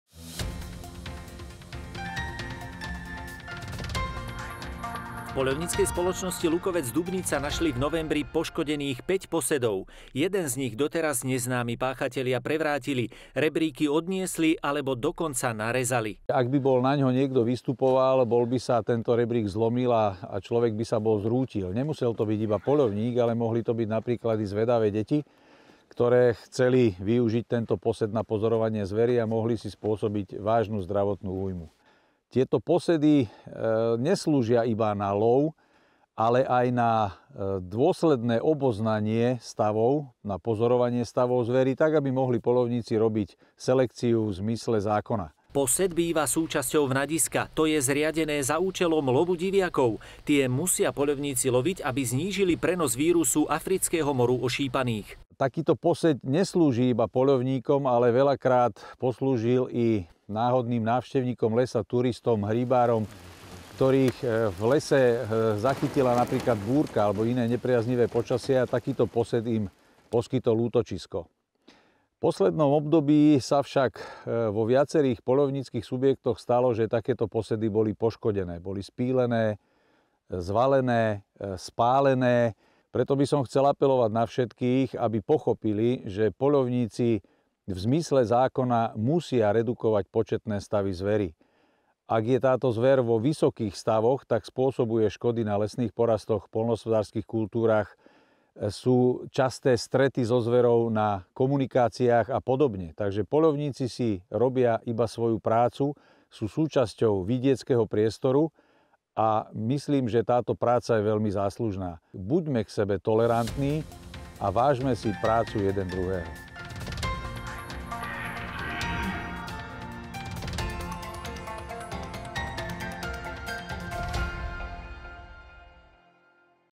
Zničené posedy
Buďme k sebe tolerantní a vážme si prácu druhého
V posledných dňoch sme vás informovali o podaní trestného oznámenia na neznámeho páchateľa za poškodenie niekoľkých posedov okolo vnadísk. Odvtedy nám poslali fotografie zničených posedov aj ďalší užívatelia, napr. prvá fotografia vo videu je zo Sološnice.
Posed v mnohých prípadoch neslúži len na cielený lov, ale aj pozorovanie zveri, sčítanie, monitoring, či obyčajný úkryt pred nepriaznivým počasím. Protiprávne konanie niektorých indivíduí, ktorí poľovníkov príliš nemusia, môže poškodiť zdravie a vážne uškodiť nie len im, ale komukoľvek kto sa na posed rozhodne vyliezť...
Veríme, že sa už podobné prípady nebudú opakovať.